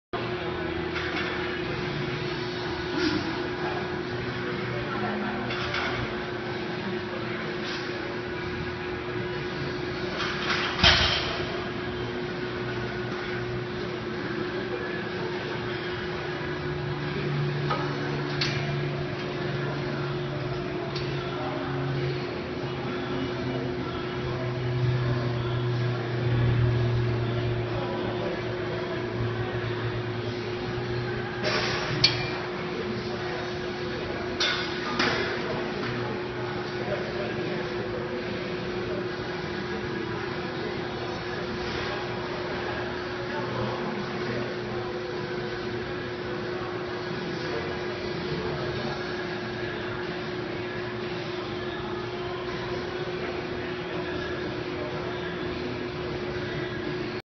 Killer lower abs workout at Golds Gym Venice
Targeting the lower abdominals. Burn out with side to side raises at the end of your sets. I usually do these between a set of another  muscle group, this way I have an active rest but I'm still keeping my heart rate up and burning fat! Kill it friends!